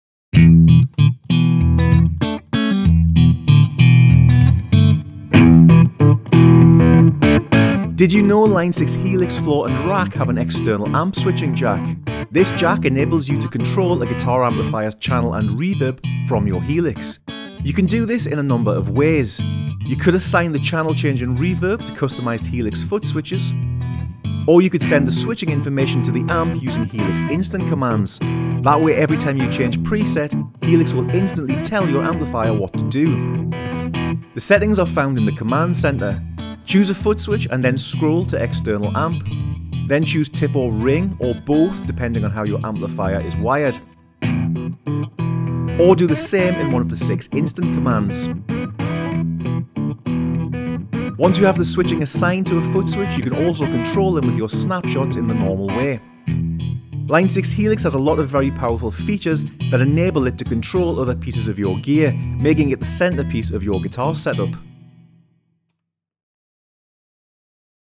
Line 6 | DID YOU KNOW? | Helix Floor and Rack - EXT Amp Switching
Did you know that Line 6 Helix Floor and Rack have an ‘EXT Amp’ switching jack. This means Helix can control your amplifiers channel change and reverb status.
Check out the video to find out more.